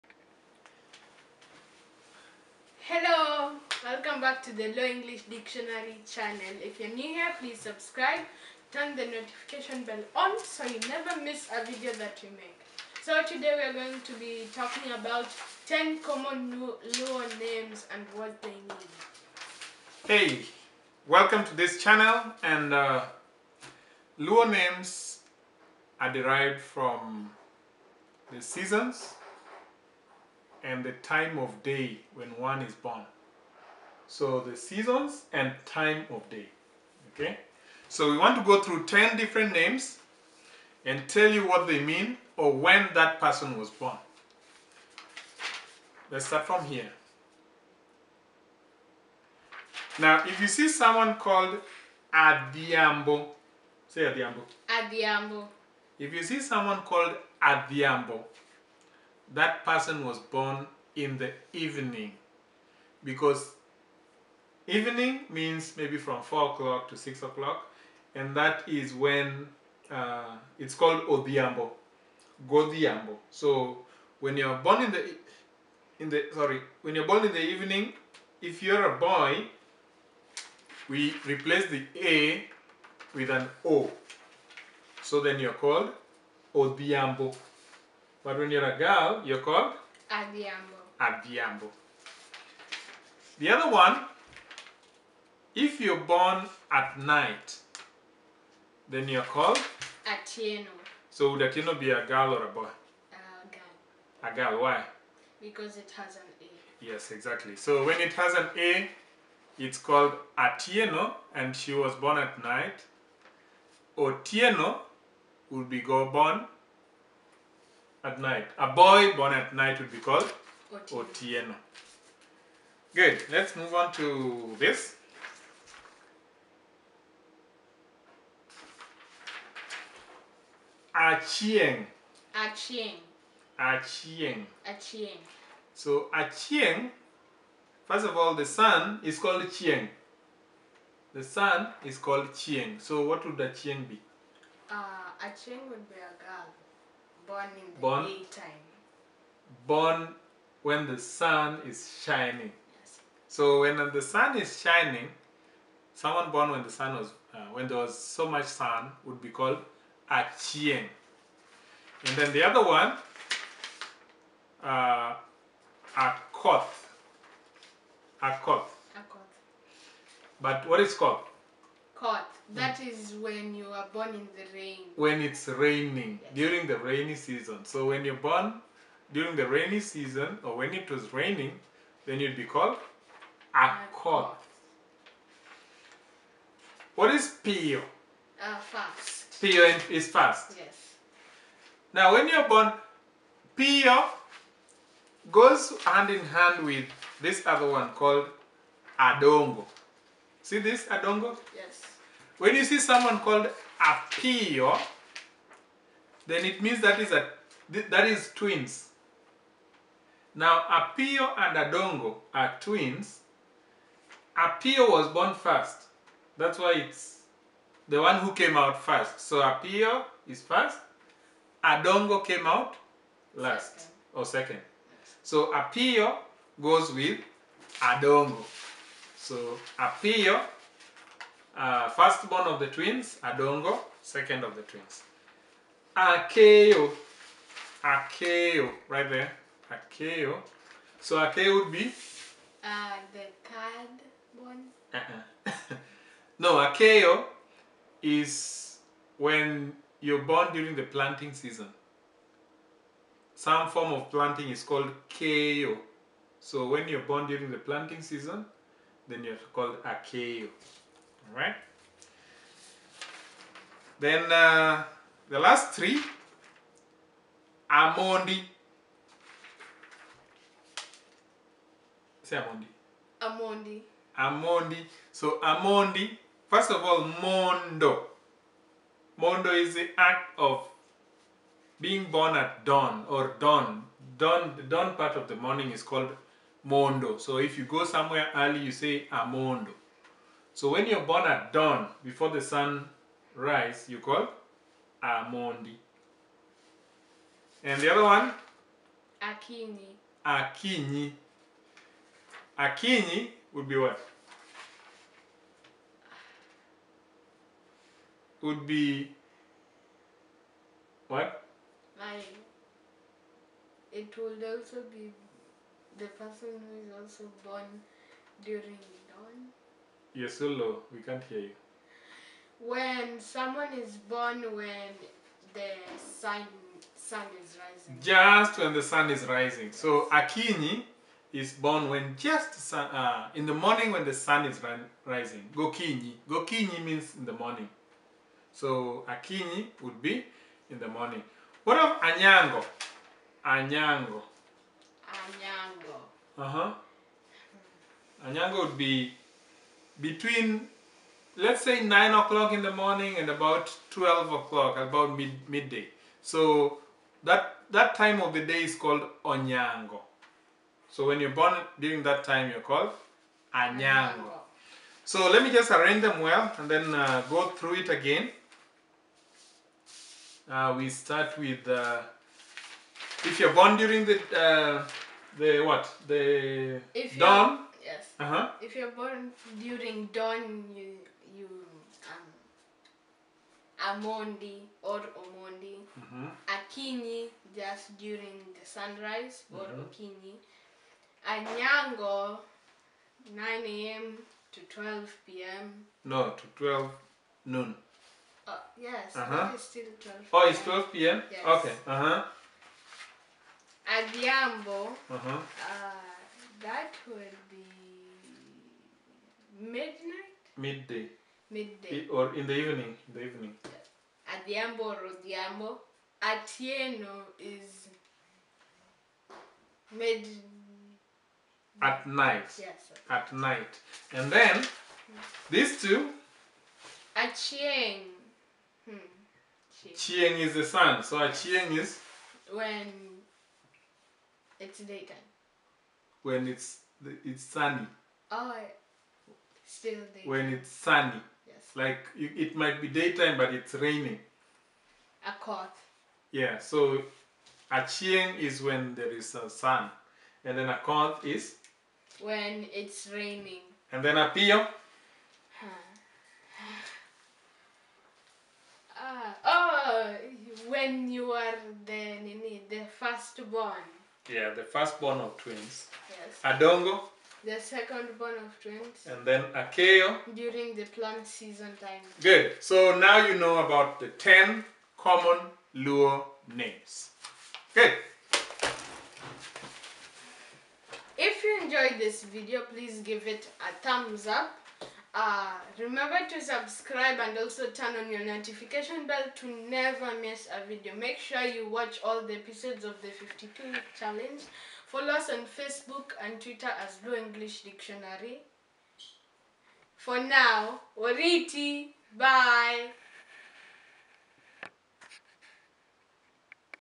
EP13 Learn 10 common Luo Names and what they mean according to Luo Language
The ten most common Luo names and what they mean. In the Luo customs, children were named according to the seasons and according to the time of day when they were born